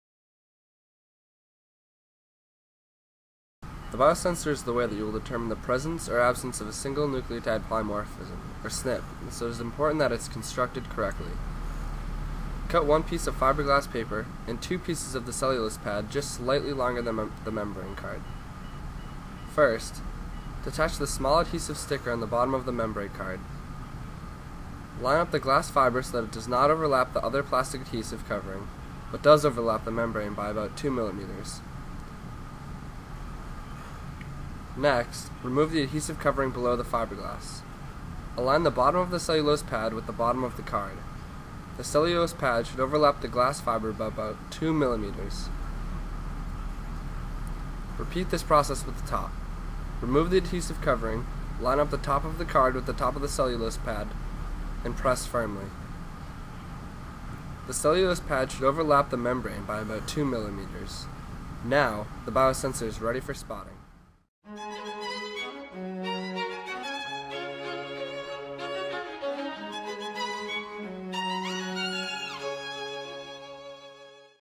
Making Biosensors
An easy format for the formation of a large biosensor strip that can later be divided and used in the maternal ancestry project.